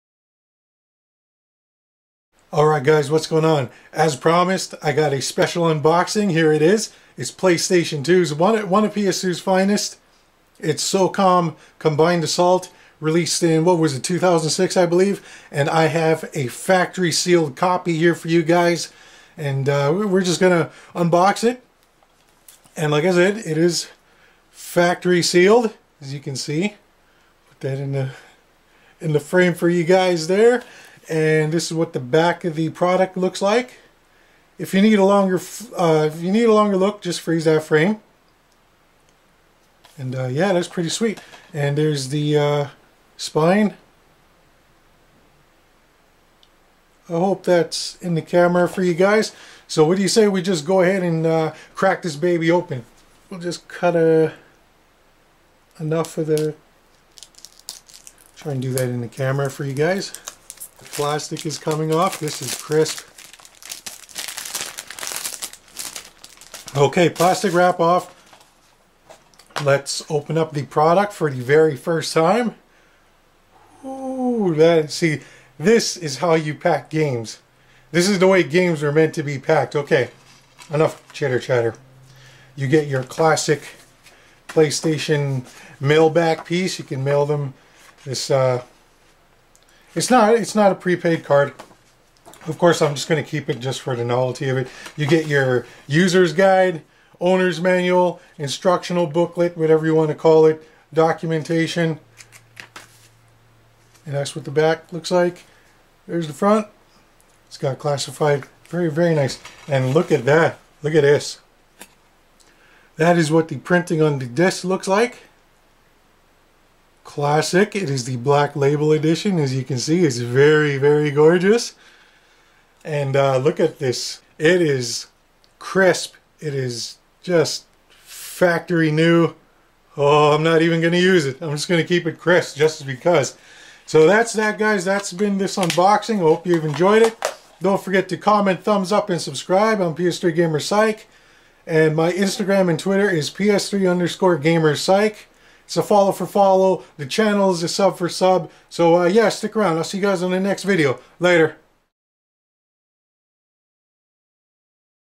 Socom Combined Assault Unboxing (PS2)
in this video I Unbox and showcase my copy of:

SOCOM Combined Assault for the PlayStation 2 gaming system.